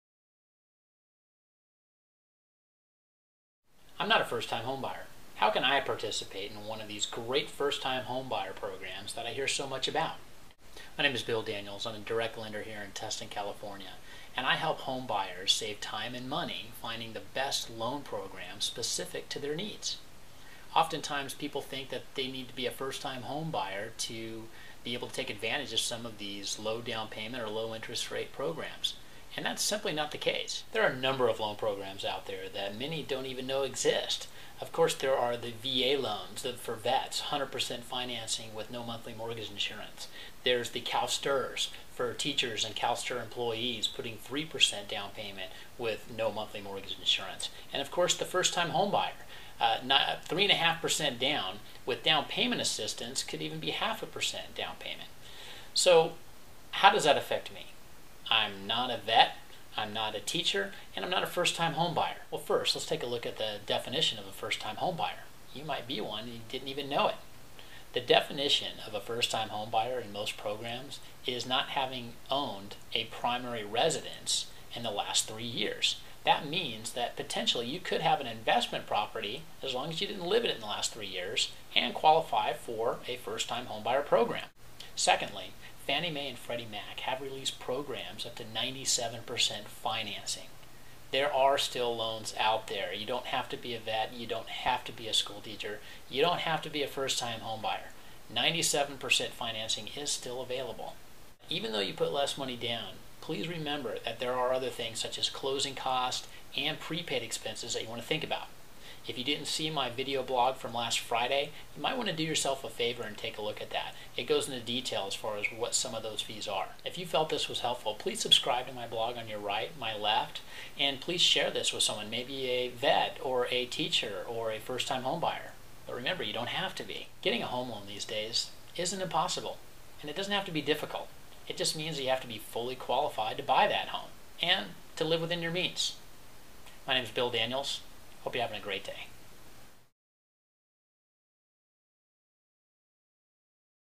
You Don't Have to be a First Time Home Buyer for this...
Bill T. Daniels - Every Friday I release a video blog on real estate or helpful tips on loan financing. In this video, even if you are not a first time home buyer... you can still get a low down payment mortgage and may qualify as a first time home buyer 'even if' you have owned a home in the past.